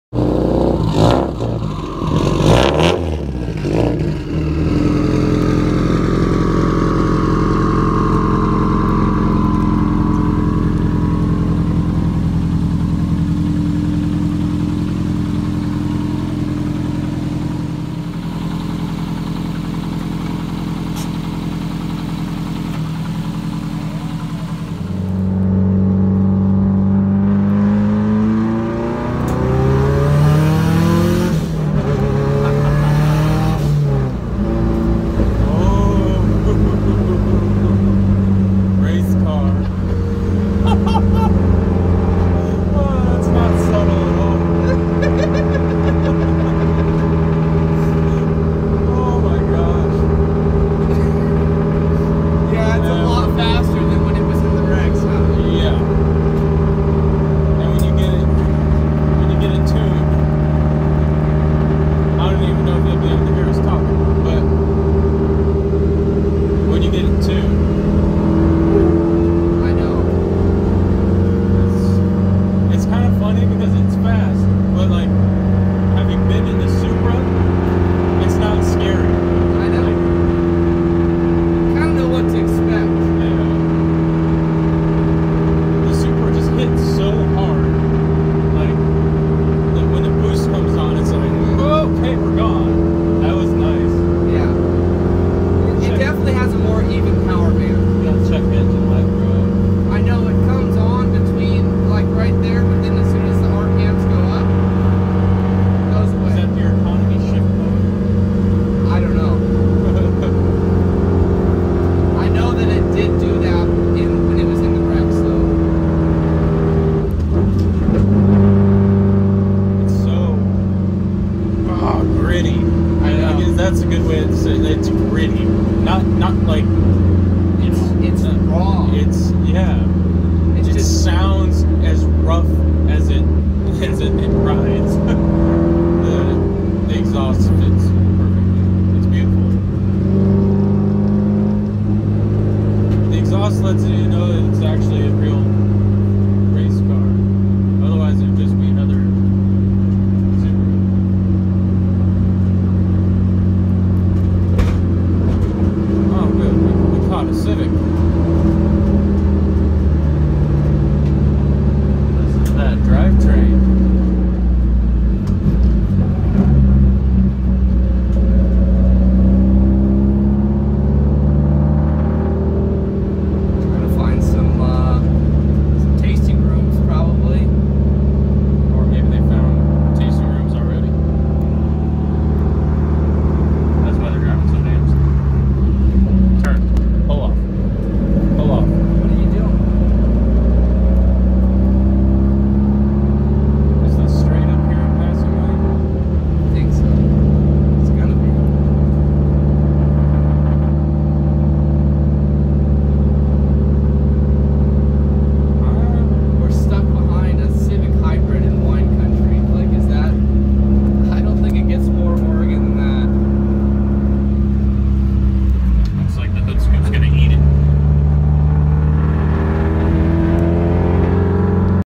Impreza RSTI Racecar
My First ride in Nic's Subaru RSti! Camera died when we got stuck behind the civic, but there will definitely be more videos to come!
Gc8 platform Impreza with a complete 06 WRX STI drivetrain swap. Ball bearing turbo stage 2 build by Horizontal Motorsports. Equal length headers.